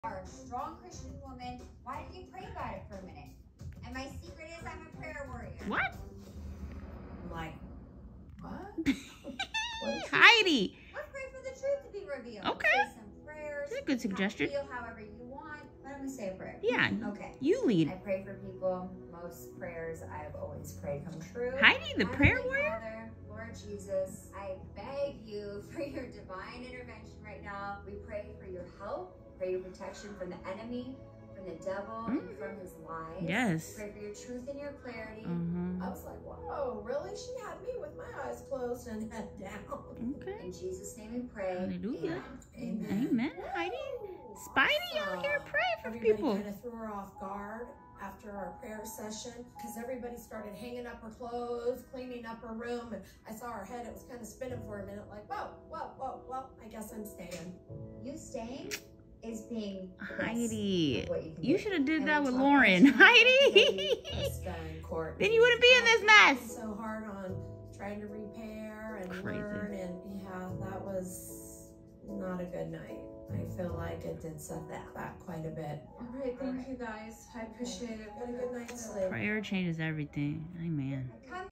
BritThompson Commentary | HEIDI MONTAG PRATT the Prayer Warrior, Christian Woman Y'ALL!
I NEVER saw this coming, but I love to see it! Heidi prays for Courtney Stodden's mom Krista and things begin to turn around